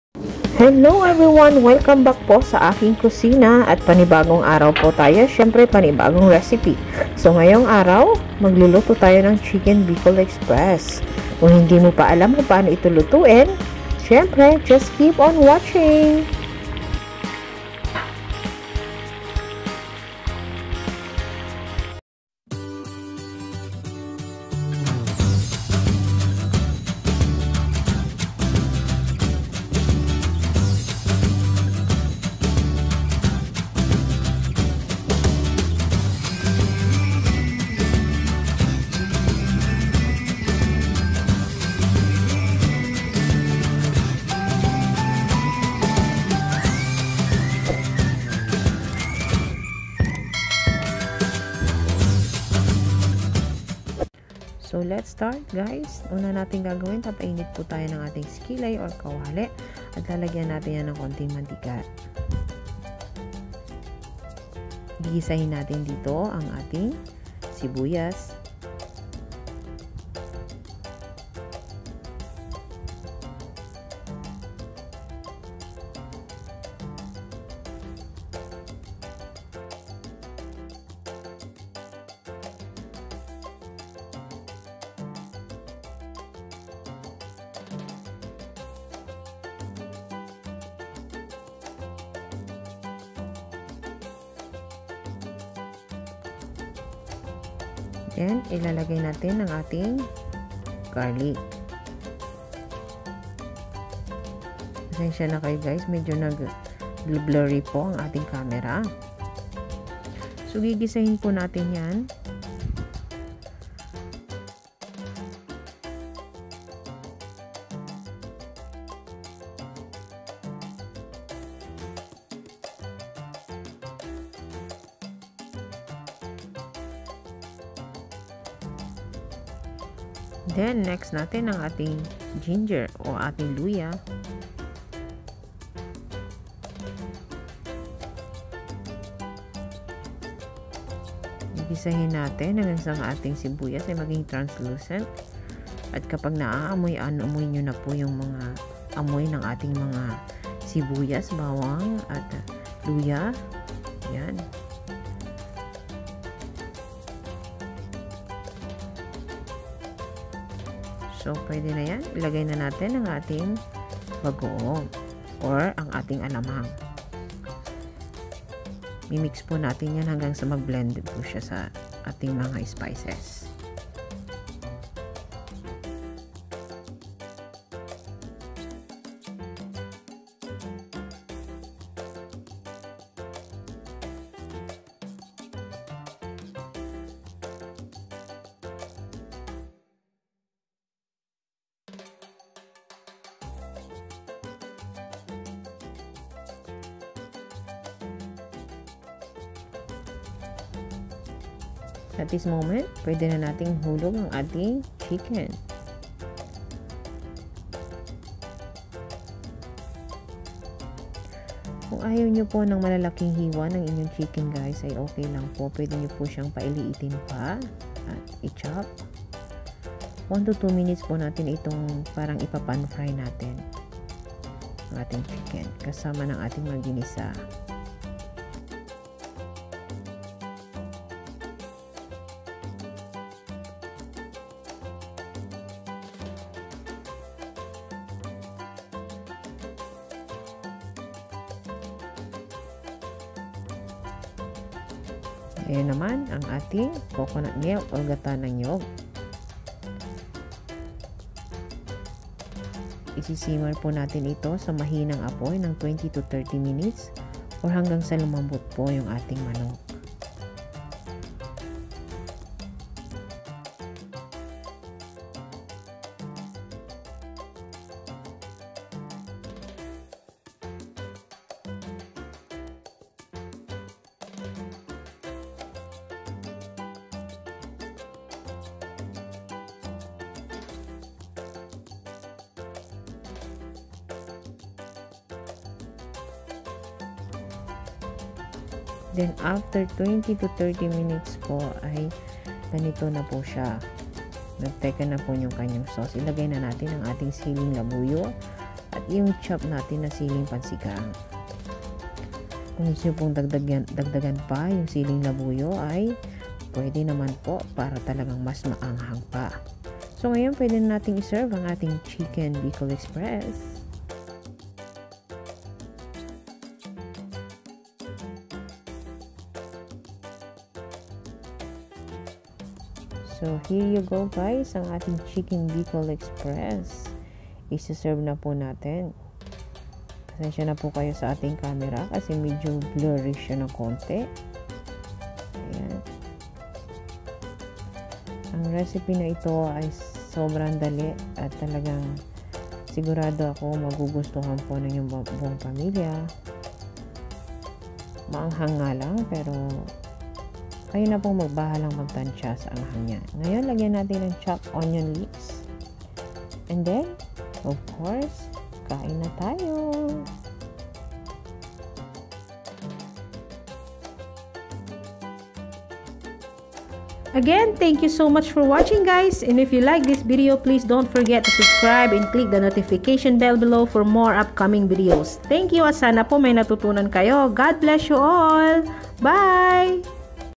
Chicken Bicol Express l Kusina Motto
Ingredients:

Vegetable oil, for sautéing
1 medium onion, chopped
5 cloves of garlic, finely chopped
30 grams ginger, peeled and sliced
2-3 tablespoons bagoong (maybe more, depending on the saltiness of your bagoong)
2-3 sili pang sigang (long green chilis)
400-450 grams chicken thigh fillets, cut into hefty chunks
400 ml coconut milk (gata)
sili labuyo, if you really want to crank up the heat!

Please watch the full vedeo for full cooking instructions.